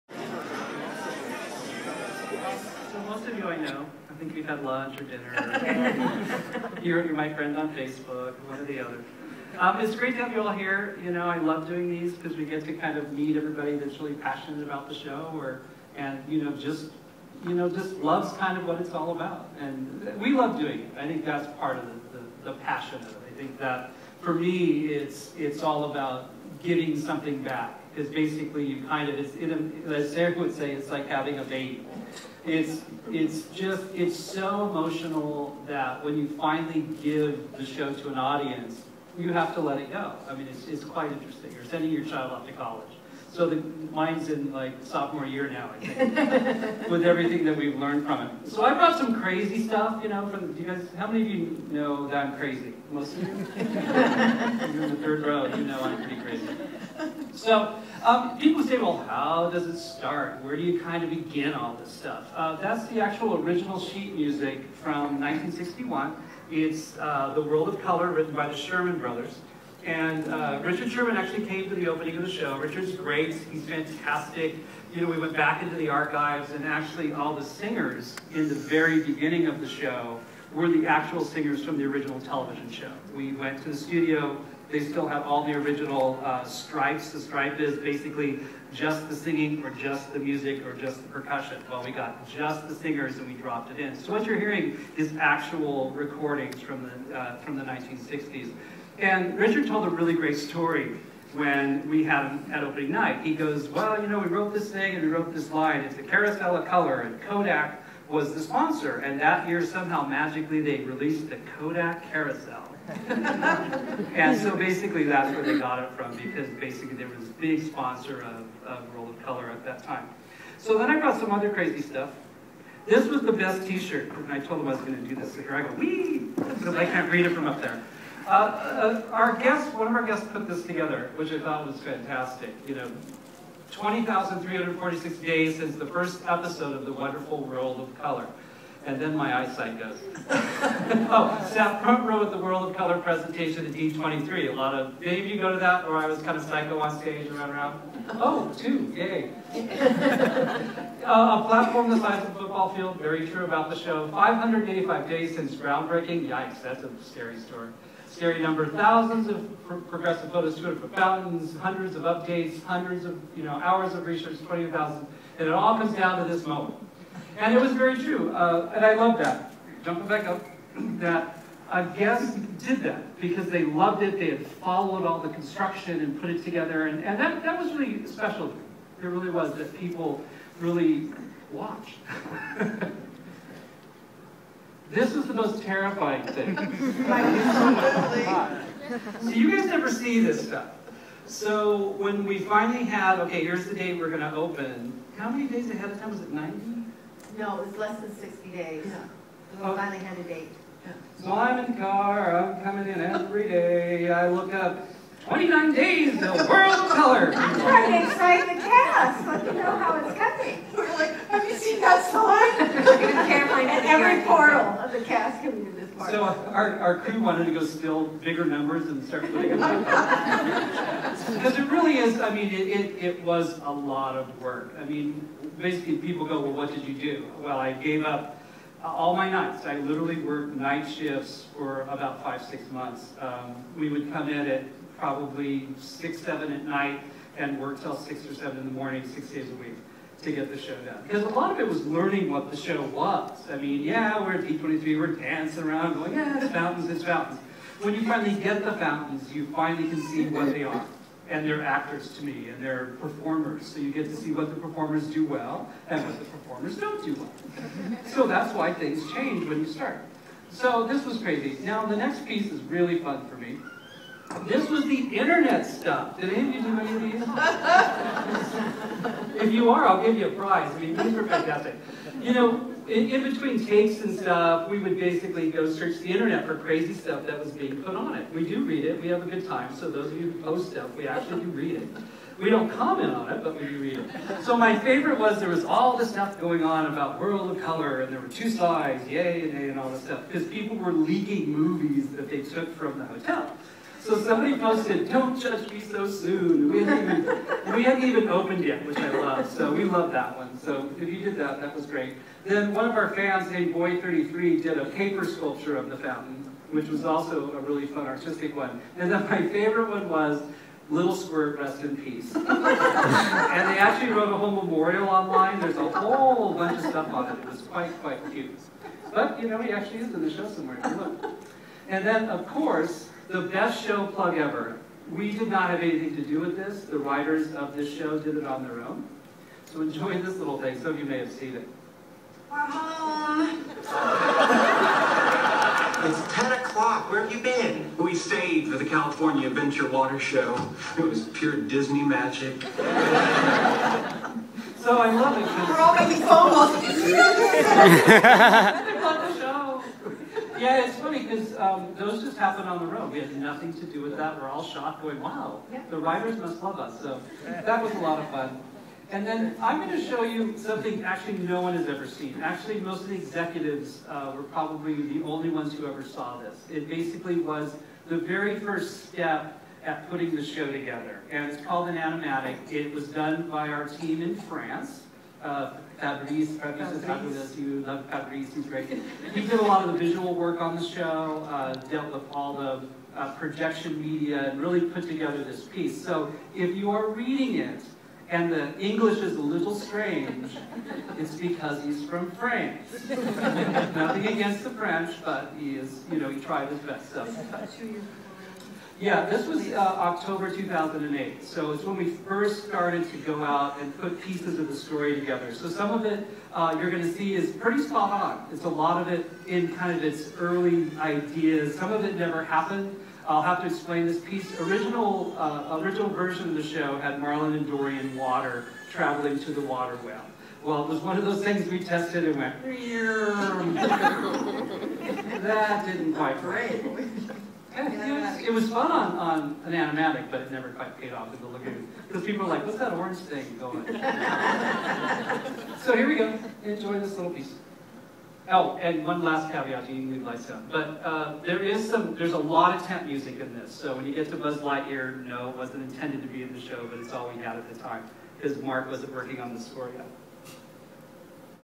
World of Color Meet-Up Part 4 of 7: Steve Davison, VP of Parades and Spectaculars
August 26, 2010 - Disney California Adventure
Recorded on an iPhone4, so I know the camera is shaky. However, the sound and image quality is still really good. Enjoy!